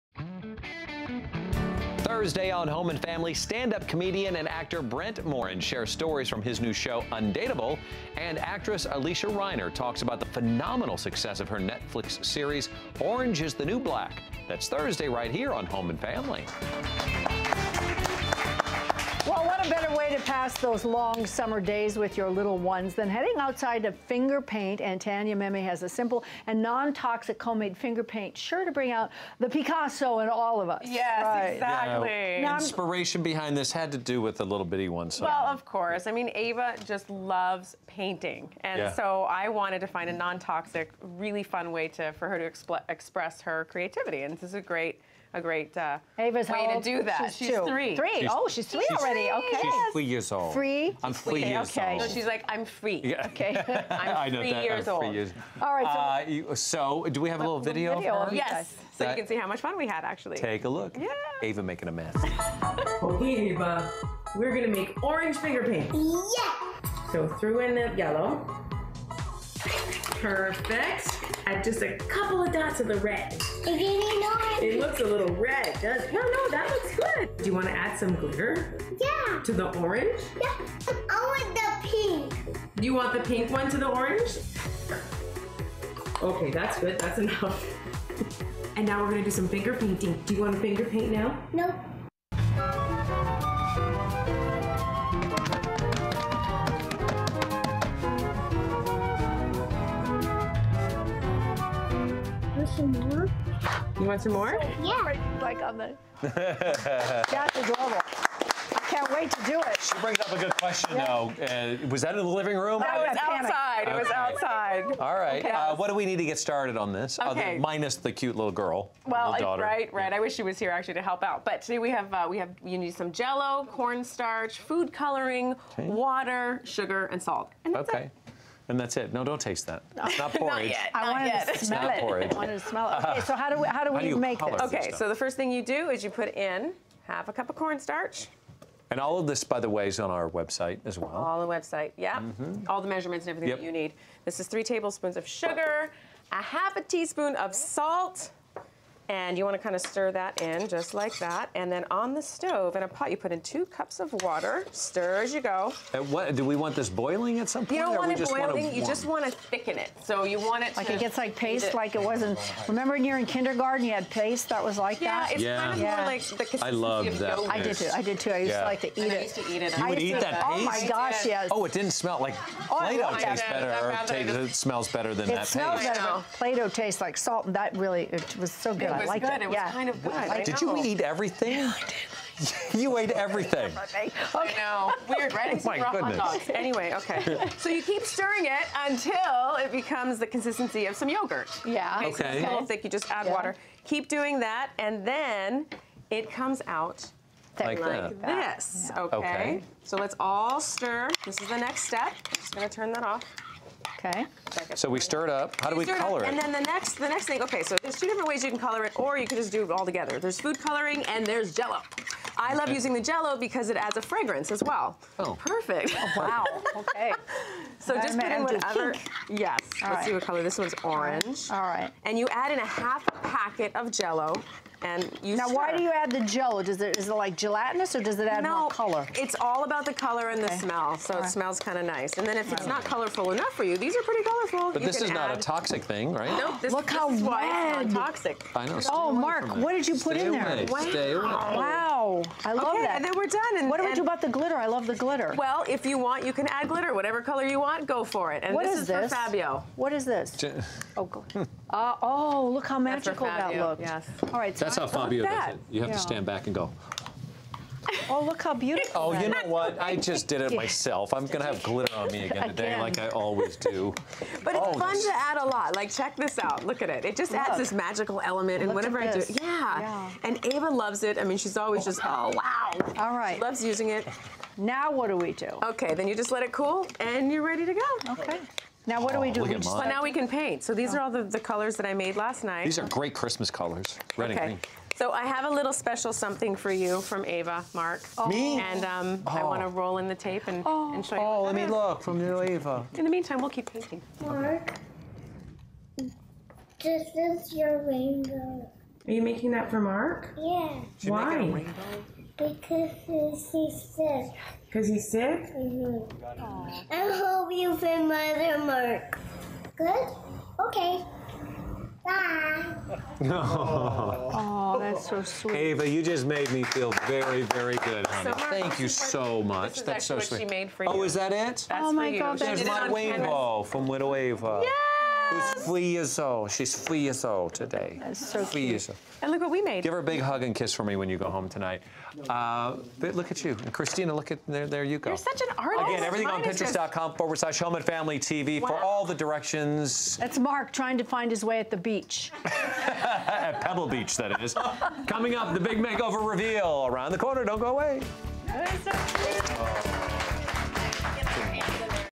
Tanya Memme DIY: How to make your own non-toxic fingerpaints for kids!
A fun & safe way for kids to express their creativity - non-toxic fingerpaints!